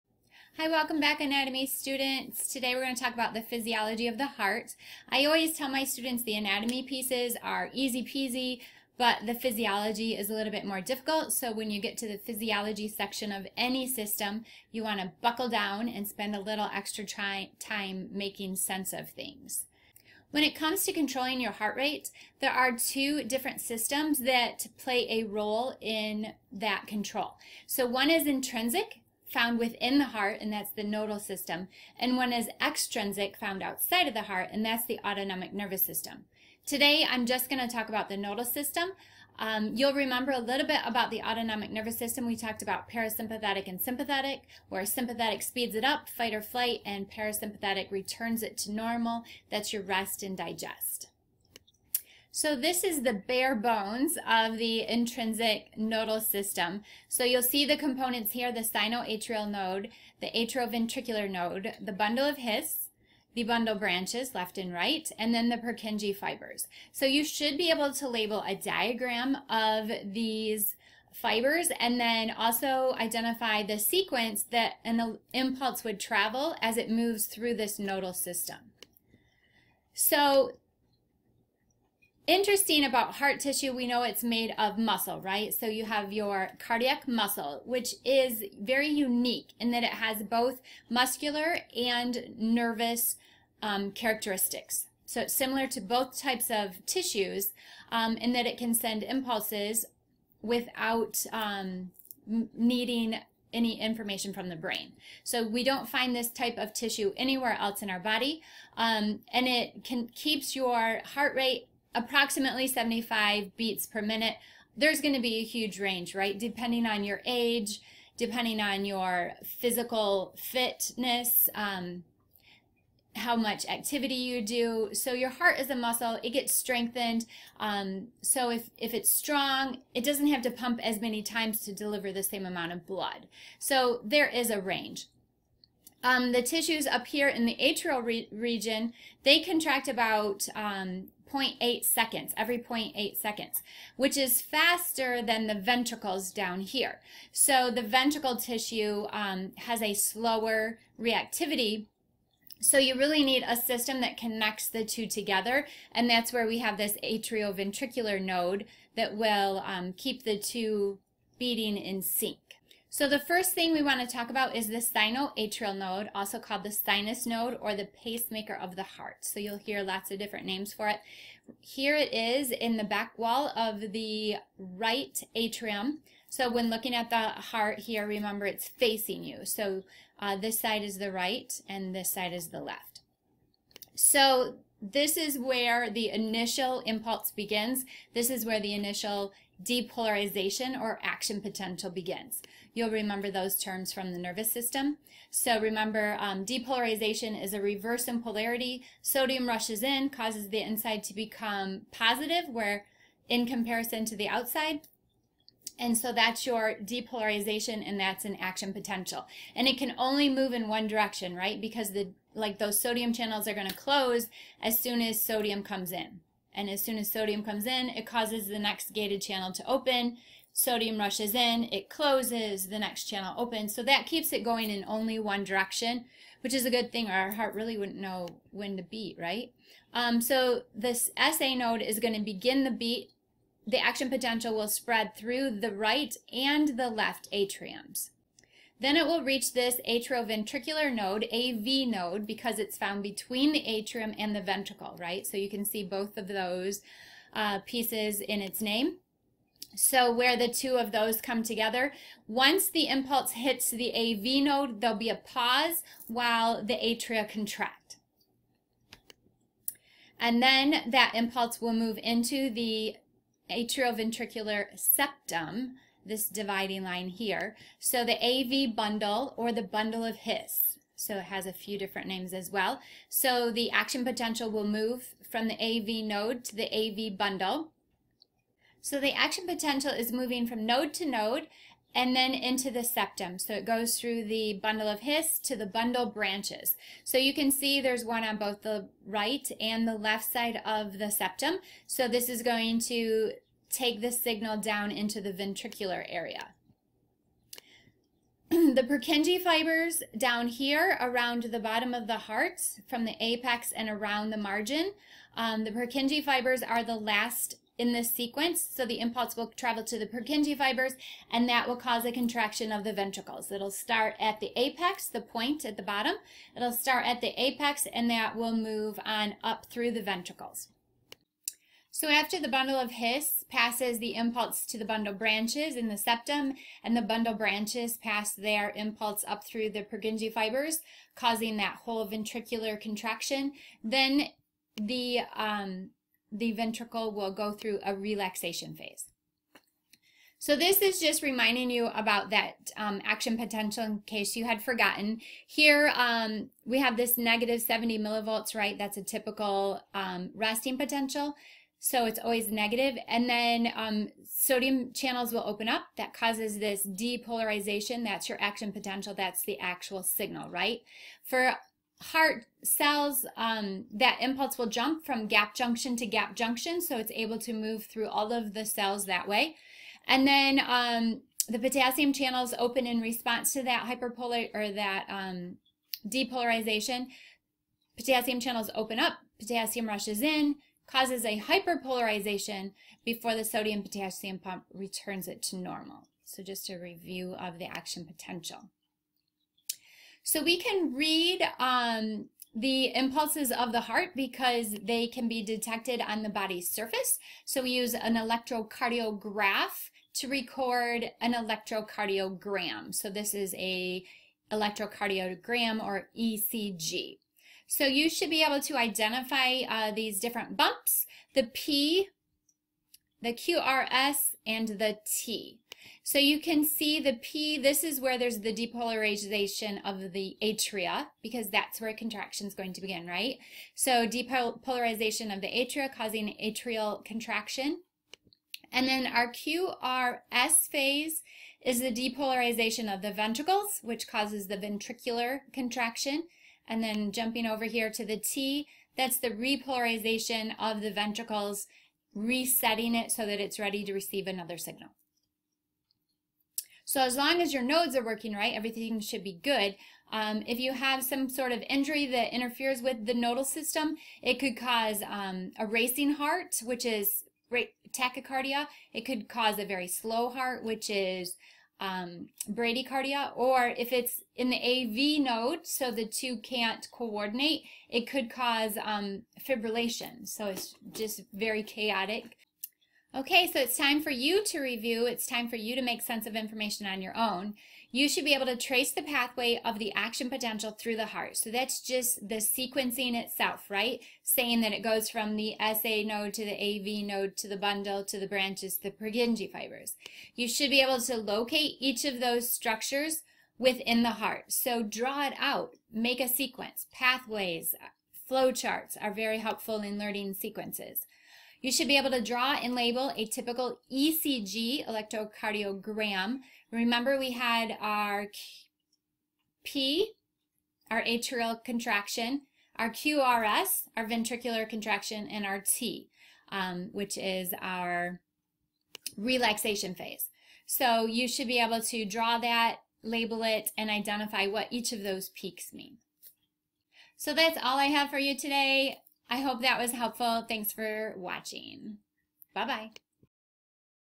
Nodal System of the Heart
This video lecture is made for a high school Anatomy class. The intrinsic nodal system of the heart is discussed along with ECGs and a review of action potential and depolarization.